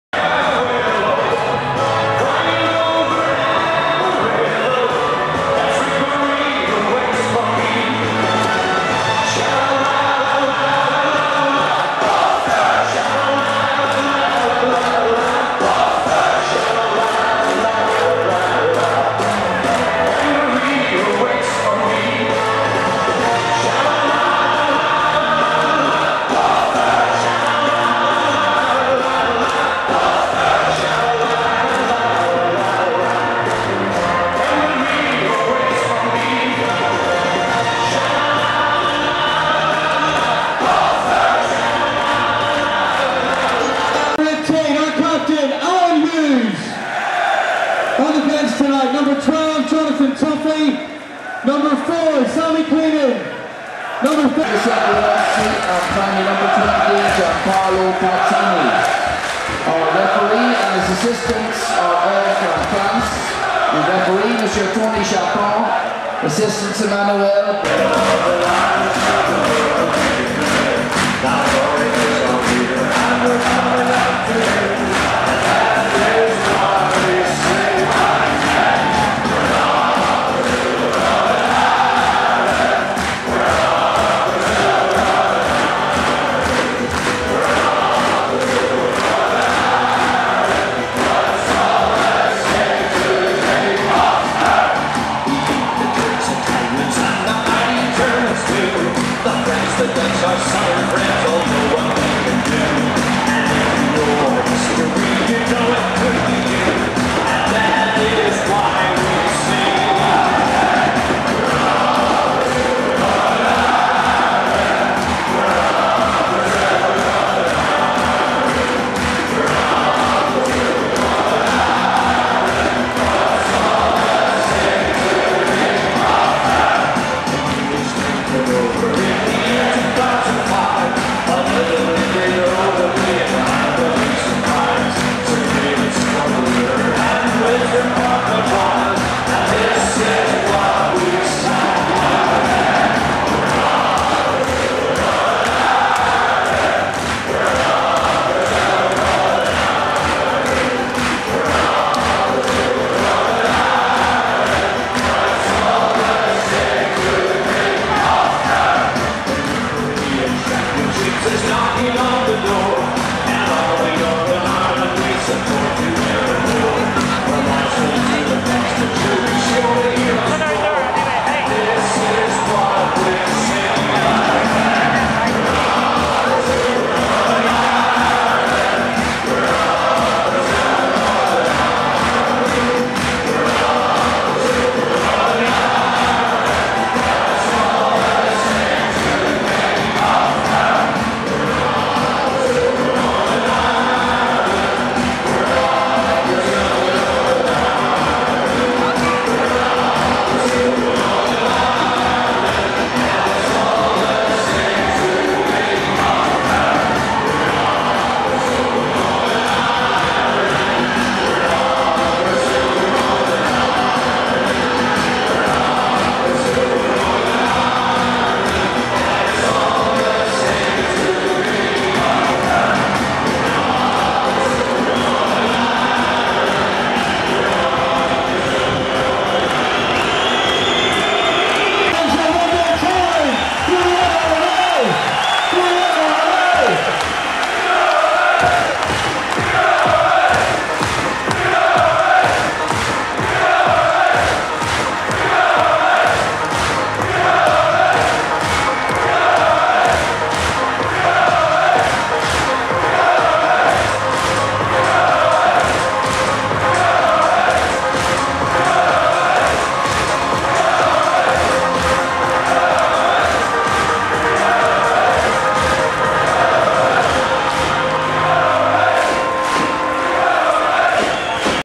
Northern Ireland fans The singing
Northern Ireland fans in full voice for the Italy match at Windsor Park Belfast Friday 8th October